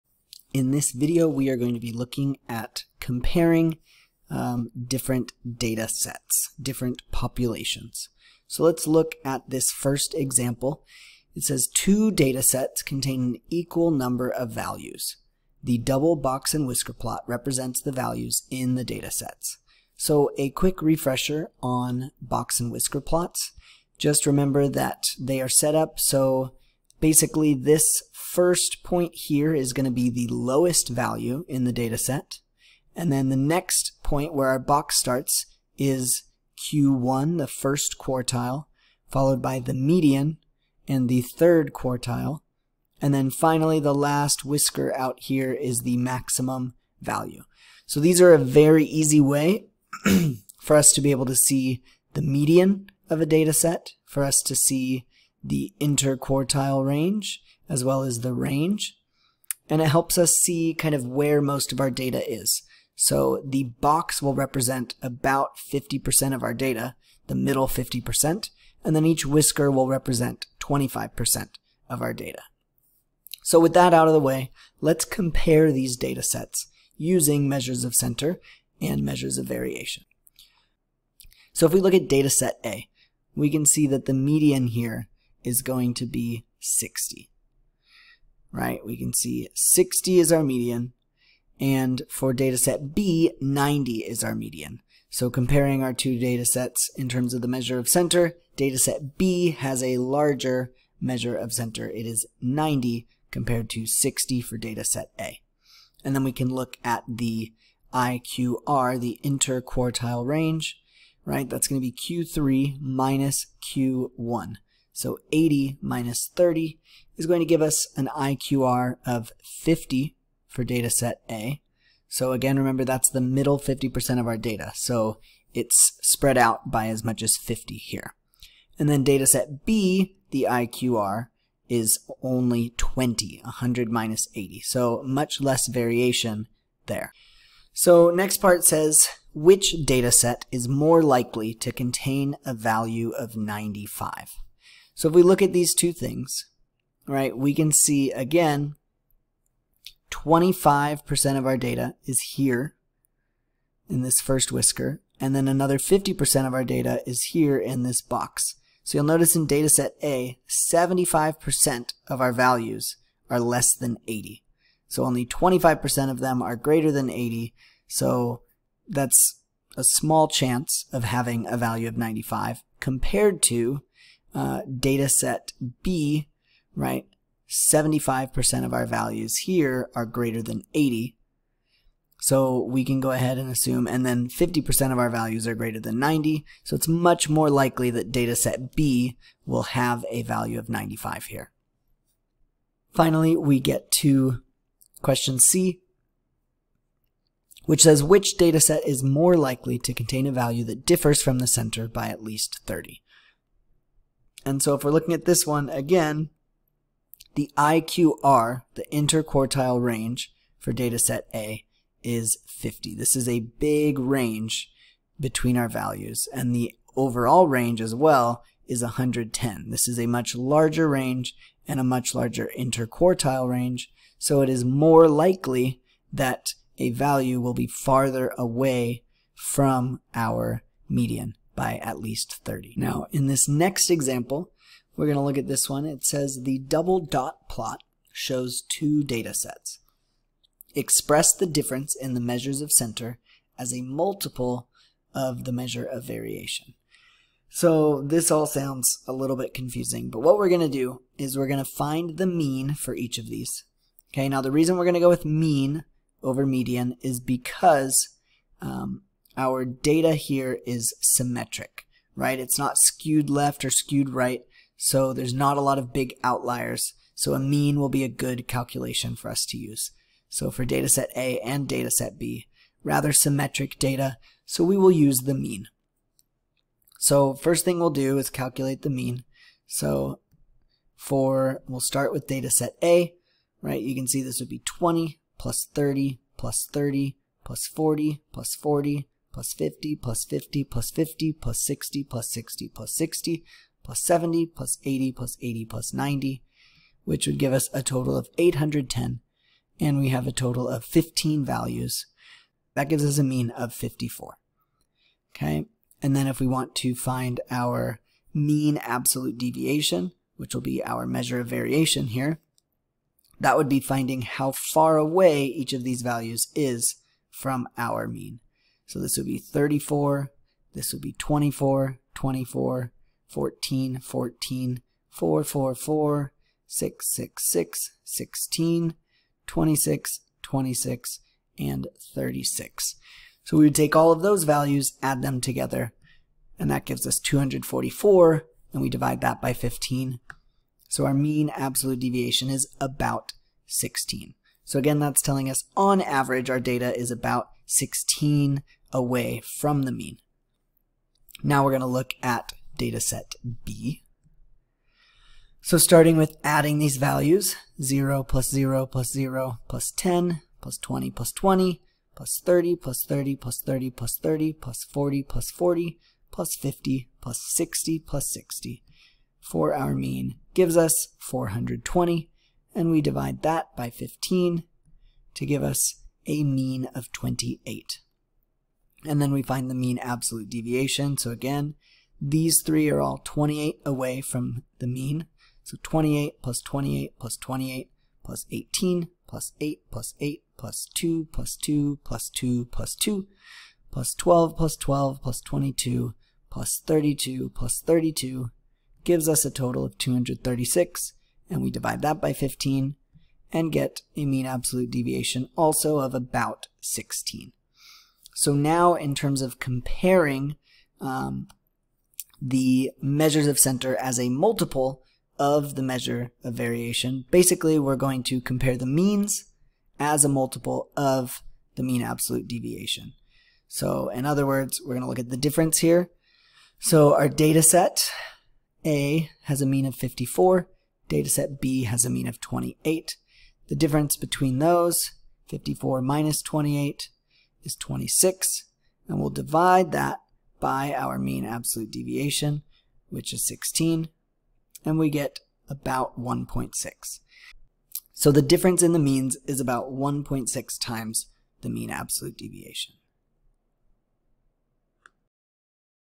Comparing Populations
7th grade math: how to compare different populations using measures of center and measures of variation